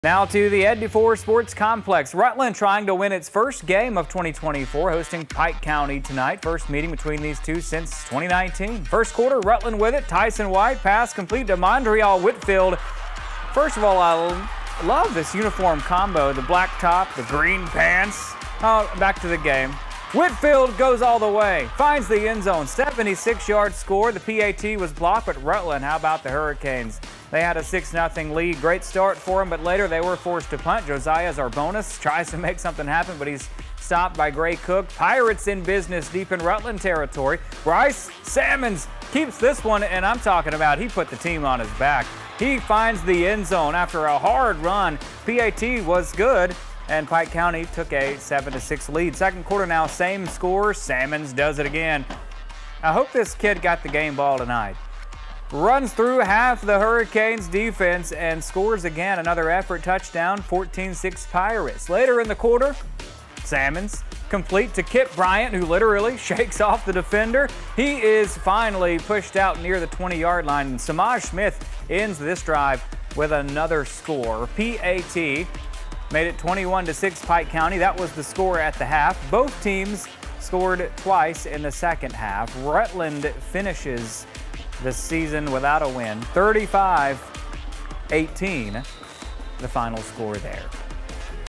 THE END ZONE HIGHLIGHTS: Rutland hosts Pike County
The Rutland Hurricanes welcomed the Pike County Pirates for the last game of the season for both teams.

Videographer: Godfrey Hall